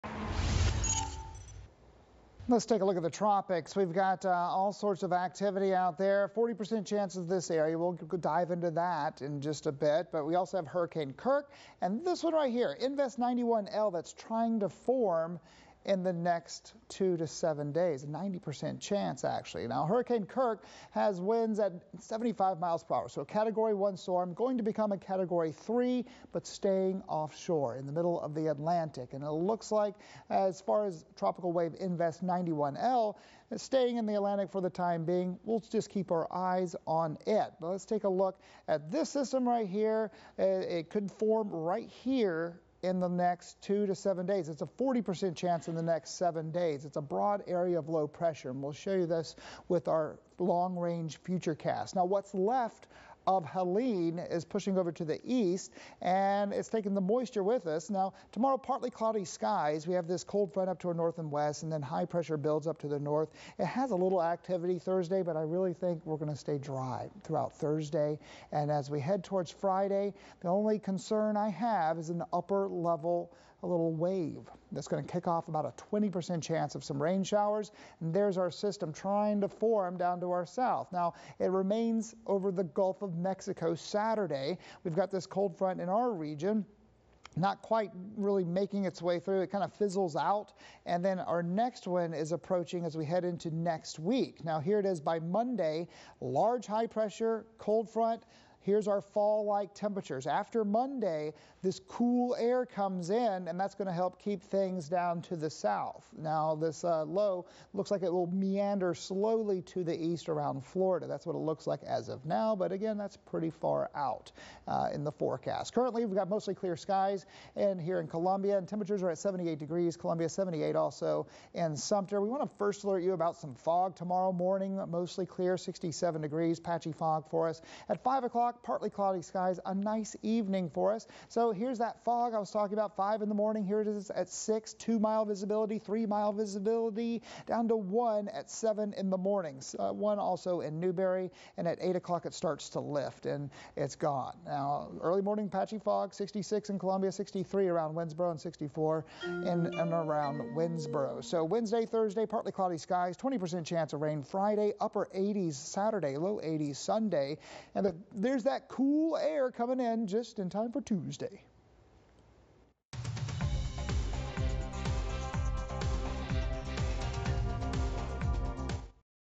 Congaree, Wateree Rivers recede below major flood stage levels
Thankfully, as of Tuesday morning, flood levels have receded below major flood stage levels.